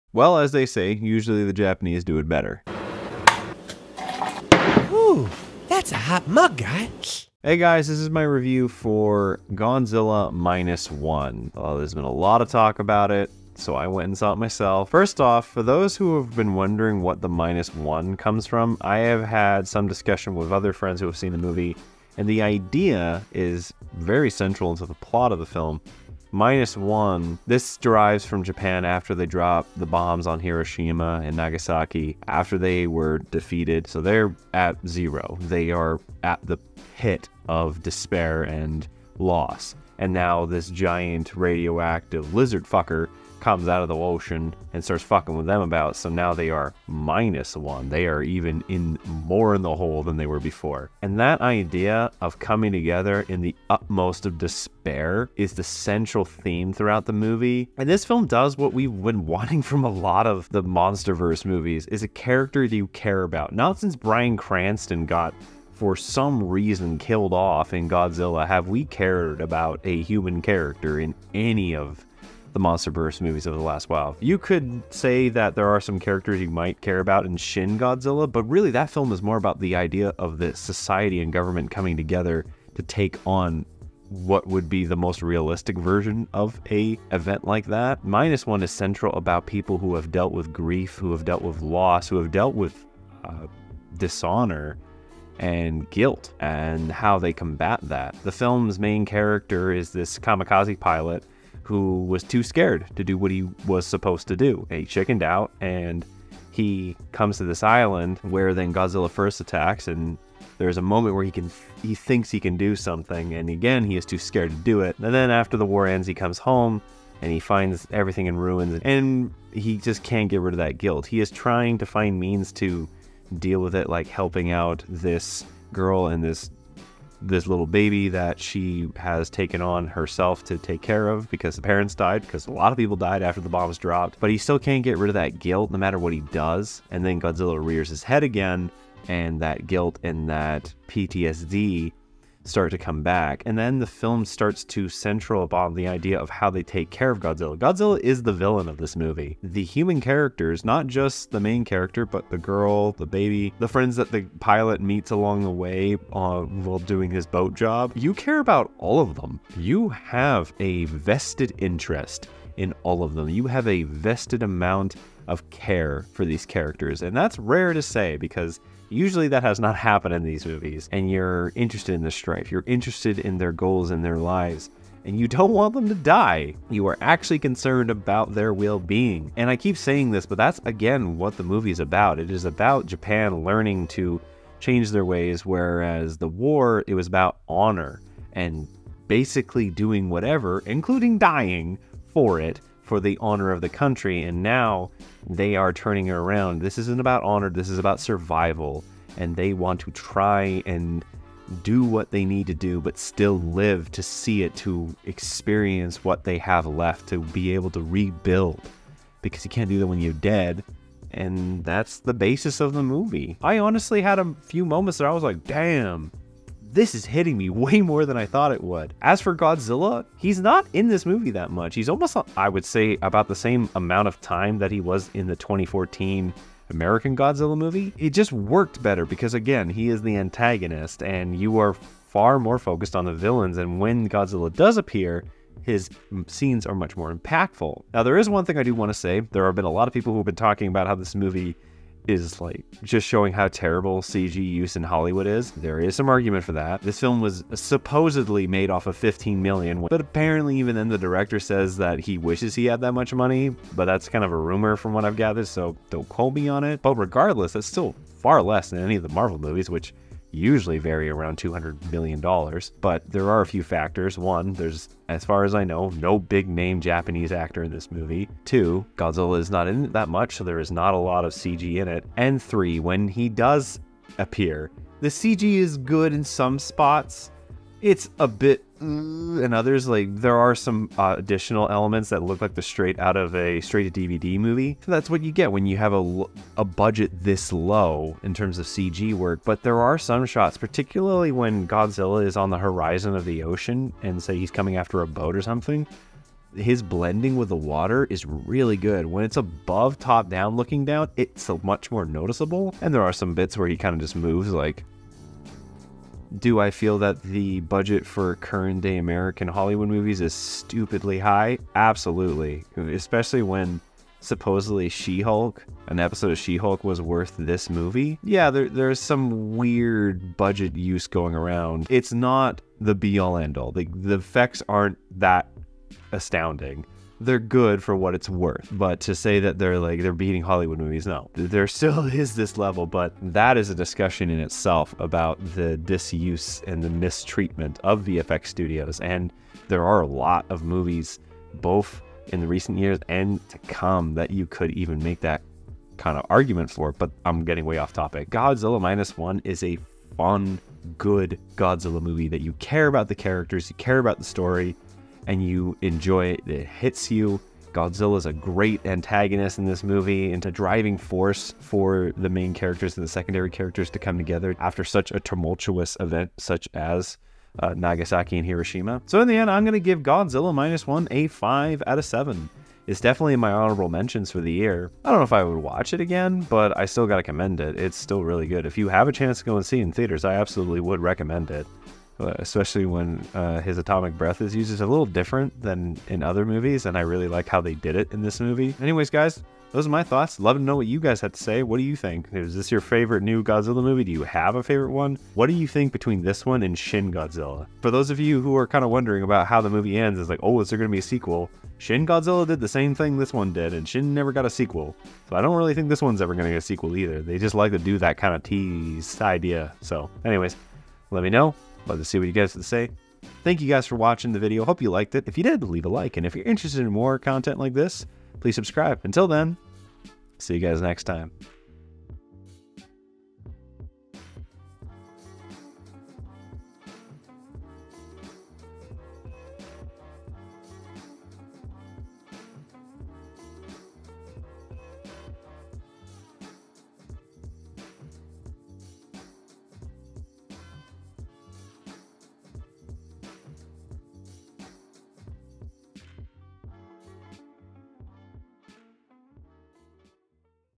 Godzilla Minus One Made Me Care
You know that one complaint all of the monster movies have had of late about wanting to care about the human characters? Well, this one covered that pretty darn good!